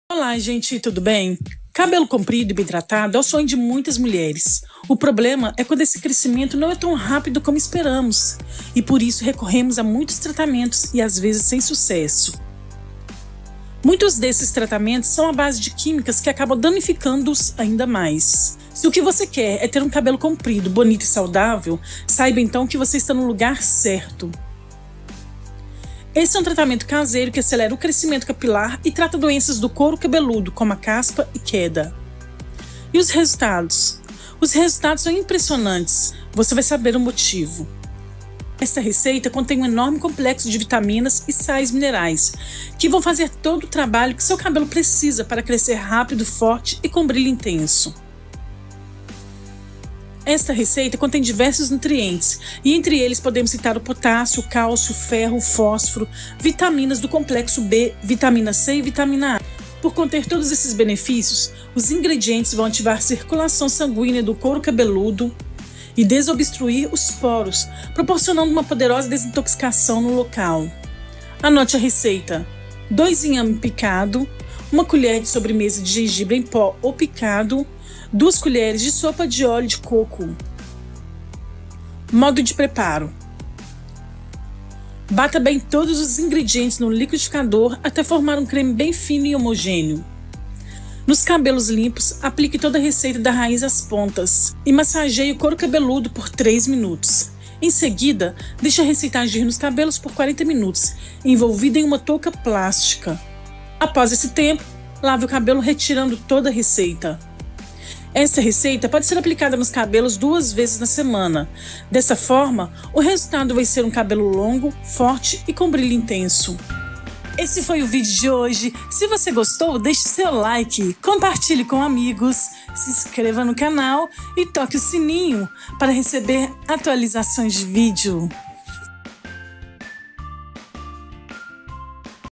Como Deixar o cabelo liso - Desmaia Cabelo
Desmaia Cabelo E Faz Crescer - Resultado Rapido Receita caseira para acelerar o crescimento capilar. O crescimento é potencializado! Vale a pena  fazer o teste, pois os resultados vao te surpreender como nunca!